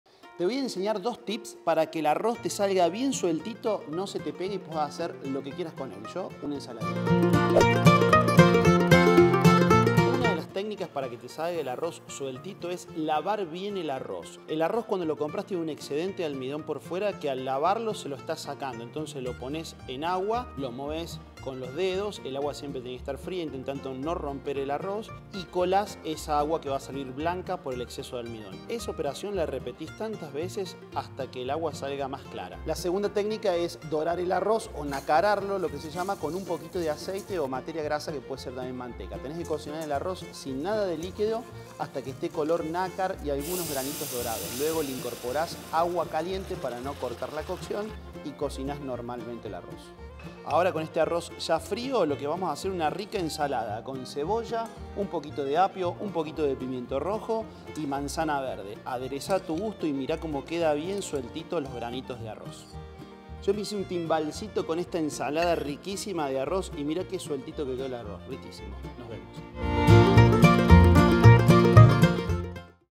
¿Arroz que no se pega? ES POSIBLE ☺️ Te damos 2 opciones para cocinarlo 🍚💪🏻
Hay dos formas distintas de hacer el arroz para que quede bien sueltito y acá te las cuenta Darío.
Todo con los más frescos ingredientes de Disco.

¿Vos cómo lo preparás?